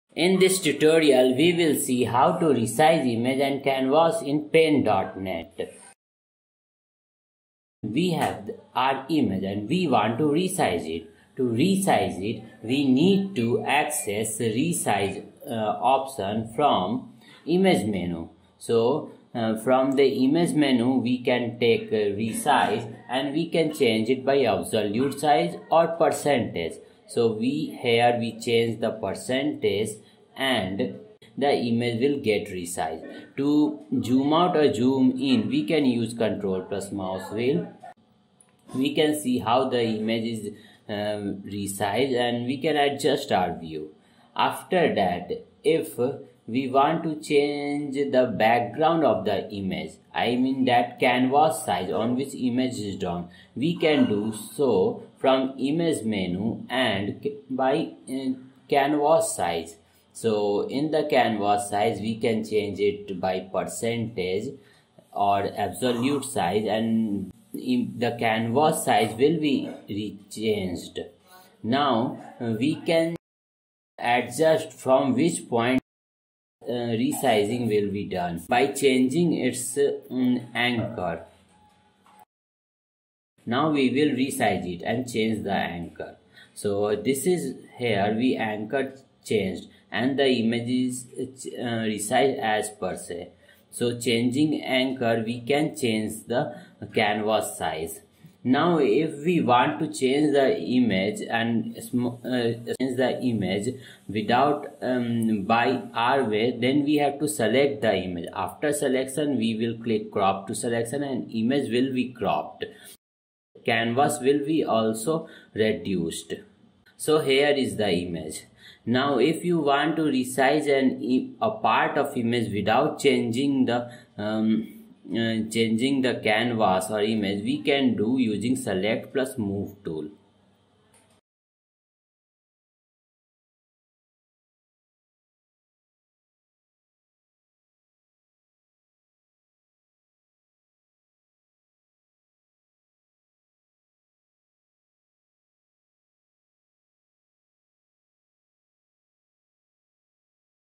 Paint.net Beginners files #5 Image resize and Canvas Resize
In this tutorial we will see how to resize an image and change canvas size